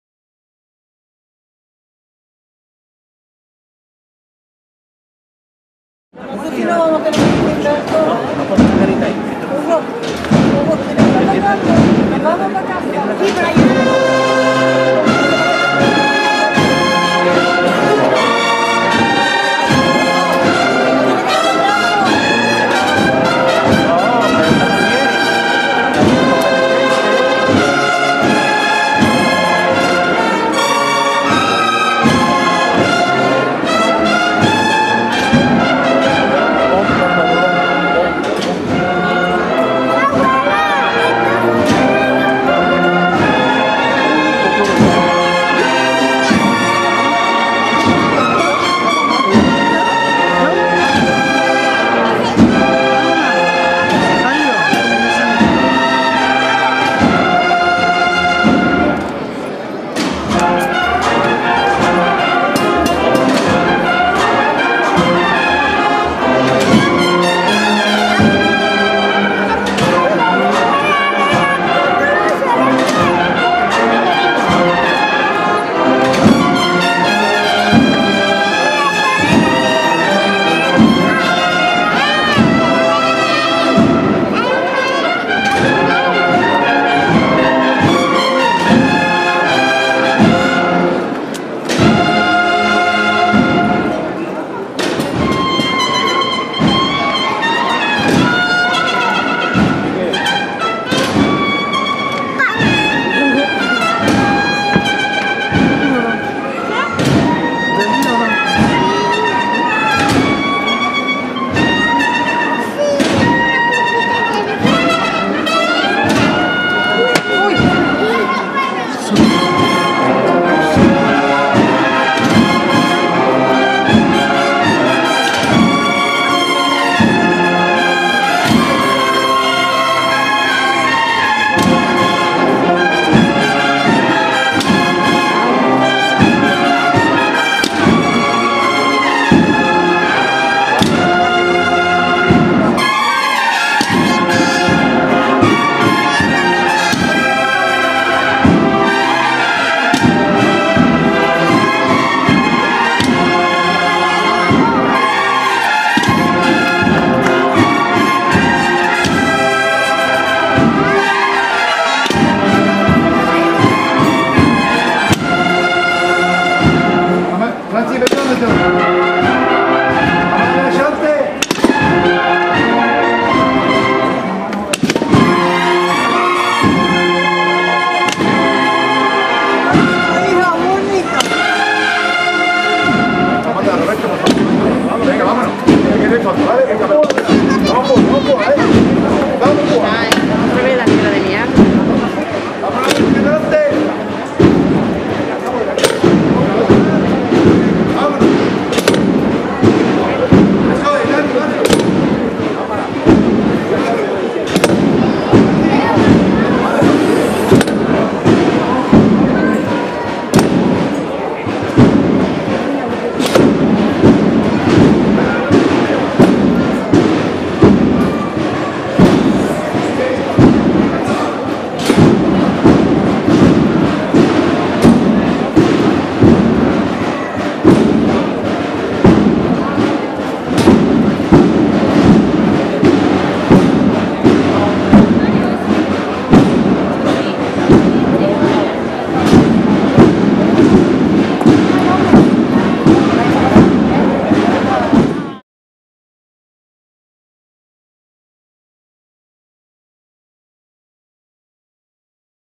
Humildad y Paciencia - Cristo por calle Novena - Semana Santa de Cádiz 2012
Santísimo Cristo de la Humildad y Paciencia por la calle Novena. Acompañado por la Banda de Cornetas y Tambores del Santísimo Cristo de las Tres Caídas de Arcos de la Frontera

Domingo de Ramos de 2012